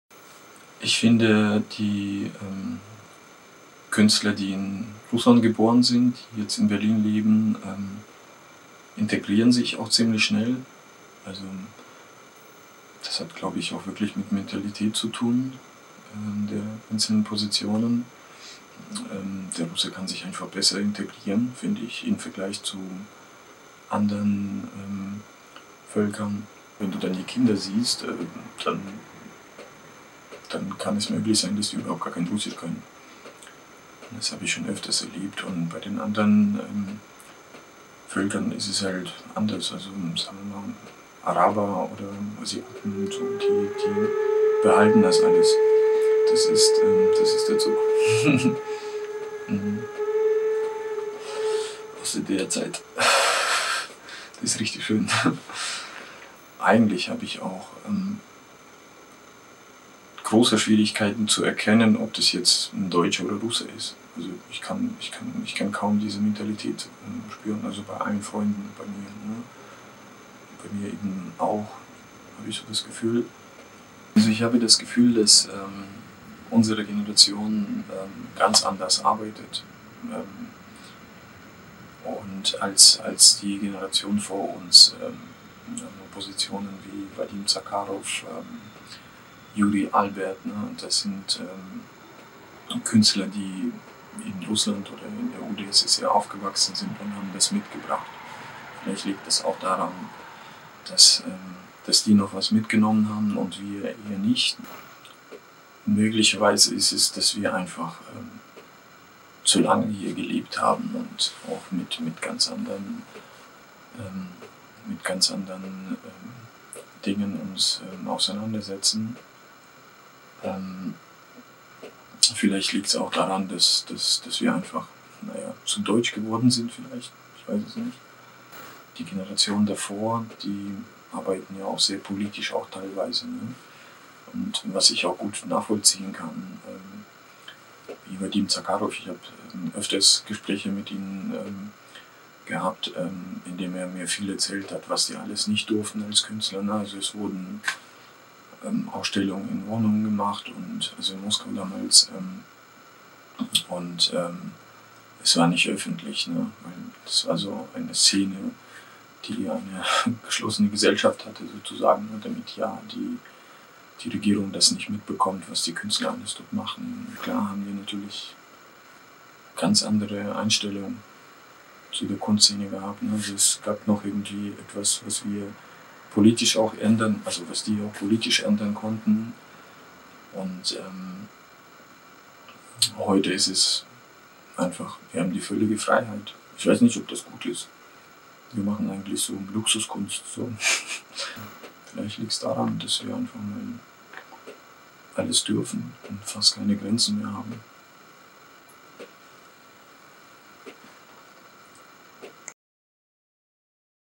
"Wir machen Luxuskunst“ – Minor Alexander über die Kunst seiner Generation | Berlinskij
"Berlinskij" hat den in Berlin und in Leipzig lebenden Künstler Minor Alexander in seinem Leipziger Studio besucht. Im zweiten Teil des Interviews spricht der in der Ukraine geborene Russlanddeutsche über die Integration russischer Künstler in Berlin und die Unterschiede zwischen der Kunst seiner Generation und der Kunst der älteren Generation, zu der unter anderem die Künstler Vadim Zakharov und Yuri Albert gehören.